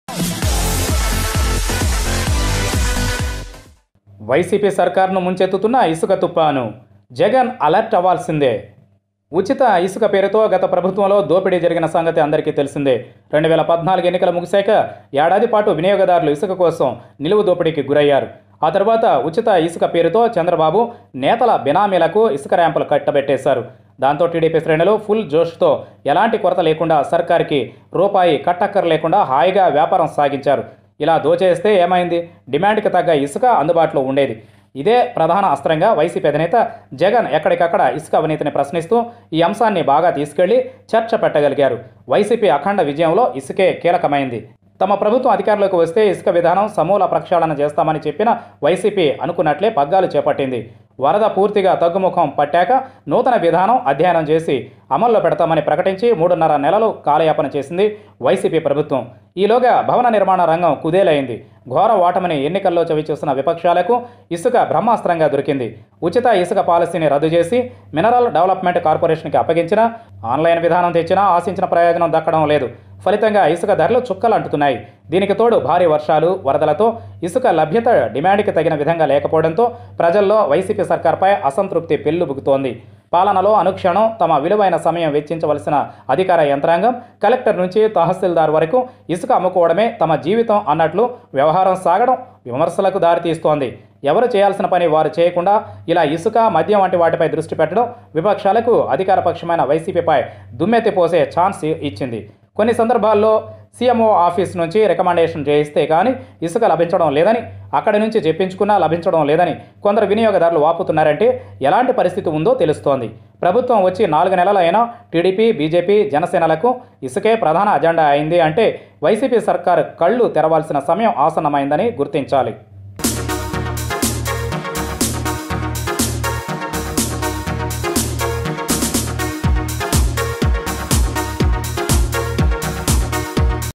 ఇసుక తుపానులో చిక్కుకున్న వైసీపీ సర్కారు| The sand Issue is a big loss for the YSRCP government
Months after the government came to be the sand is main agenda for the TDP, Janasena and BJP. It is the time to open the eyes of YSRCP Government. Otherwise, this could be a serious blow to the YCP in local elections.